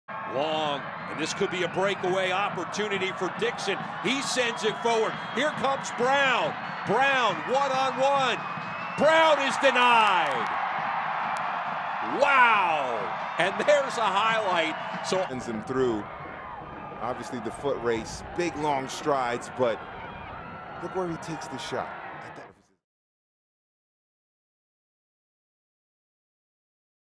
Security National Mortgage Save of the Month - September 2018
Making the first start of his Las Vegas Lights FC and professional career, hometown product Thomas Olsen kept his club in the game from start to finish vs. OKC Energy FC on Saturday, September 29, 2018. This stop on a one-on-one chance on the break stood out the most, also earning Olsen honors for the Security National Mortgage Save of the Month.